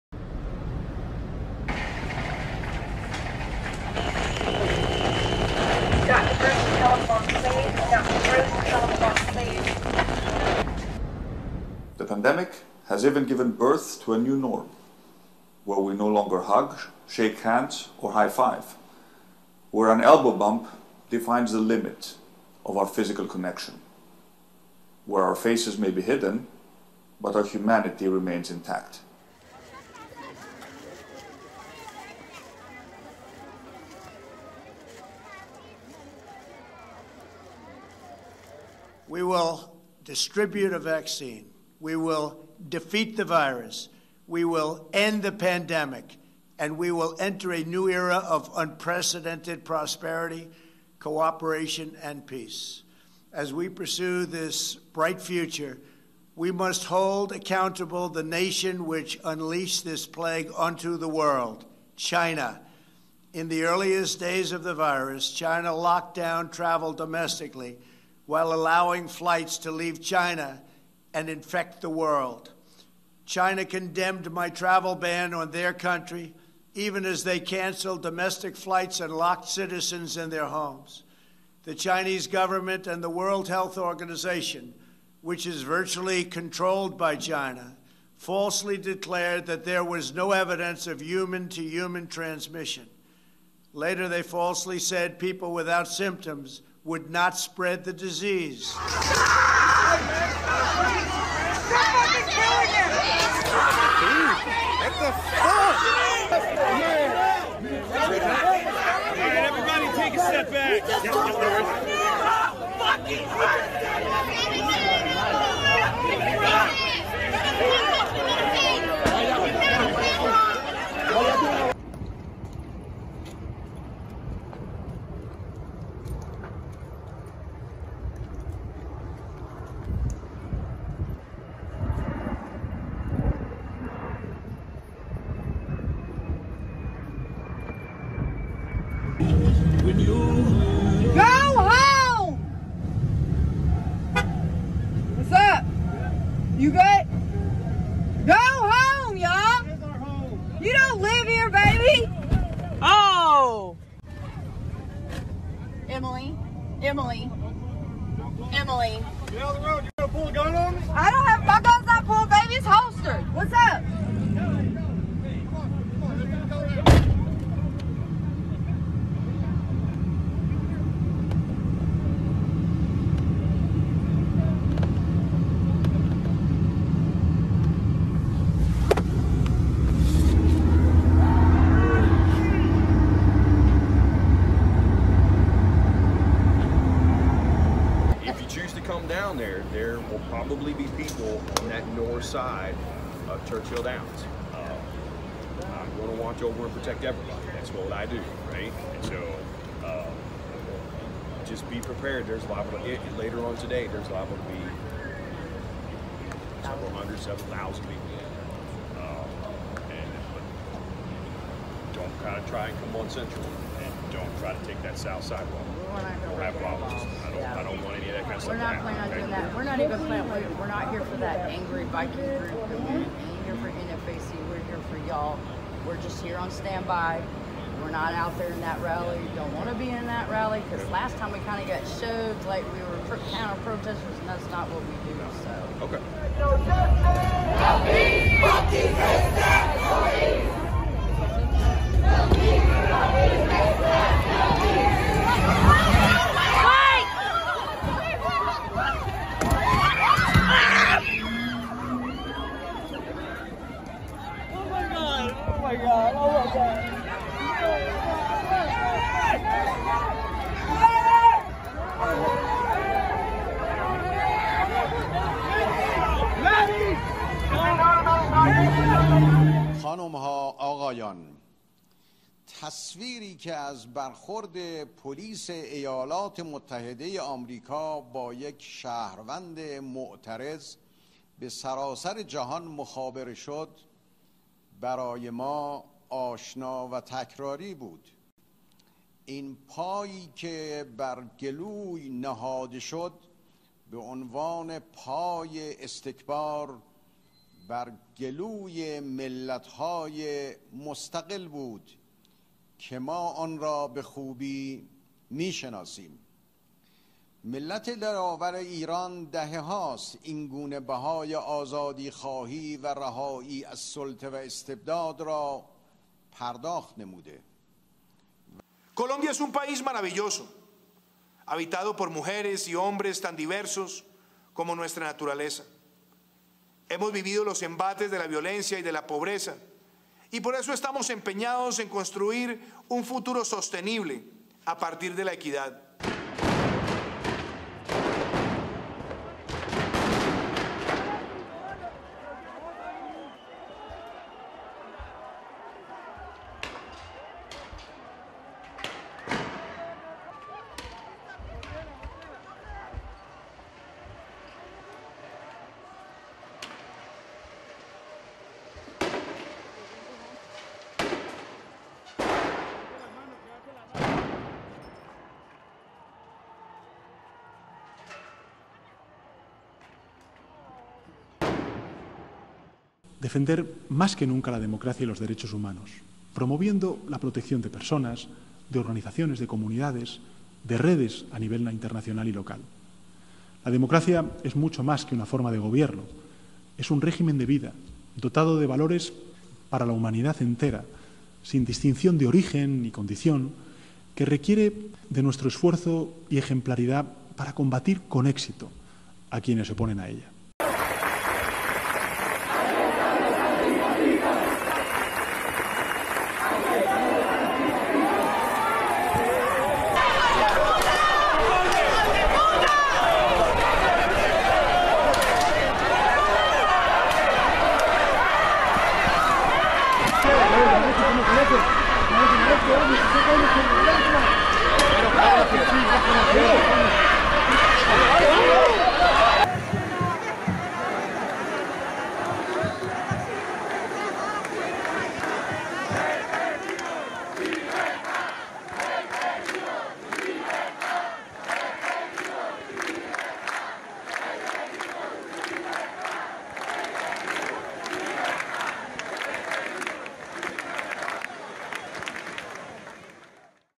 Plano Maestro. De Louisville a Vallecas, palabras y hechos
Un resumen visual de la actualidad mundial. Todo imagen, sin comentarios. Un nuevo formato de El Salto TV.